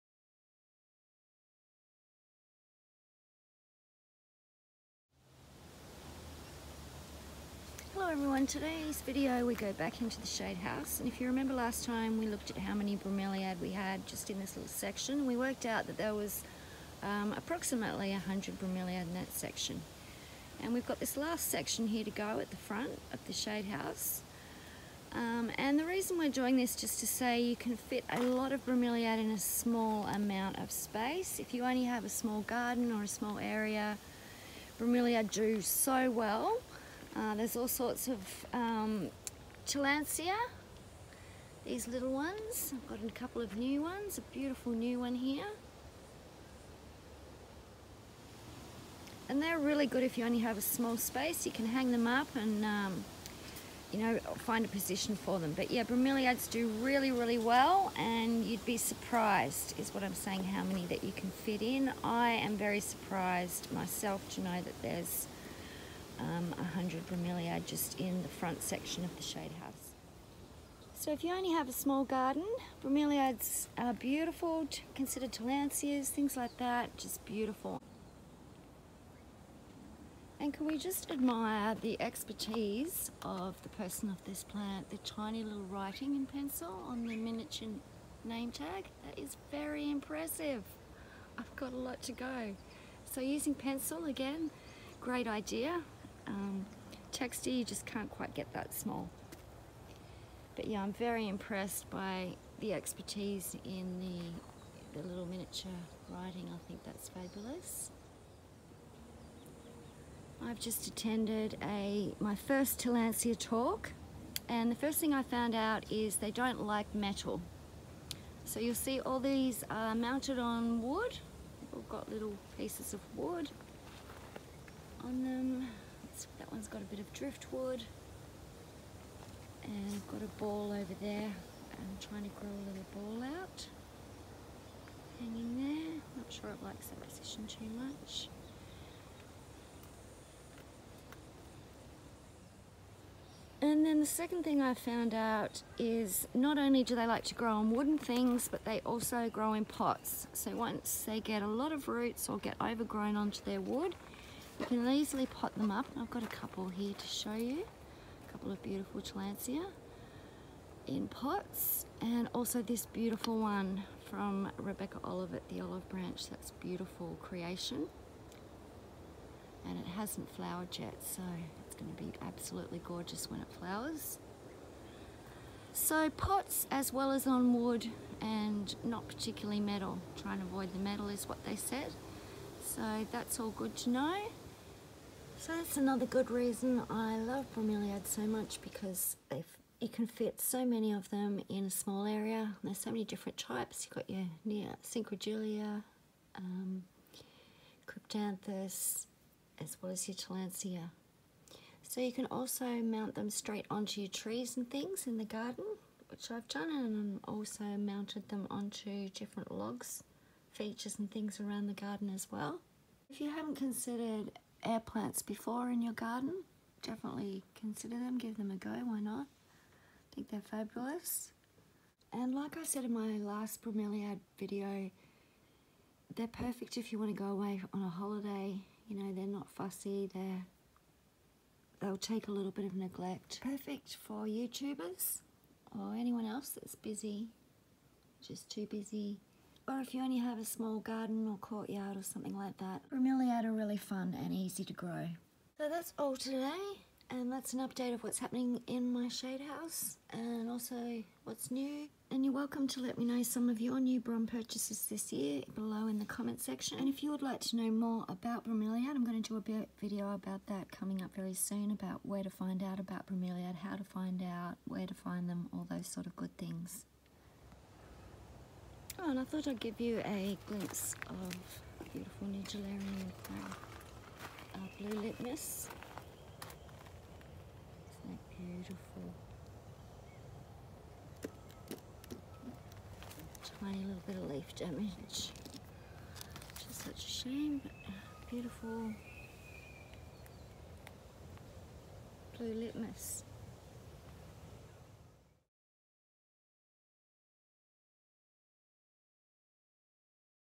TILLANDSIA BROMELIAD for SMALL GARDENS & New Plants
Ideas for Small Gardens With Tillandsia and Bromeliad plants forsmall areas and Courtyards . Find out How to grow Tillandsia and More . Take a look at whats flowering in the shadehouse October 2019 . See new bromeliad in the shade house and whats on my Brom Rack  . Air plants , Nidularuim Blue Litmus ,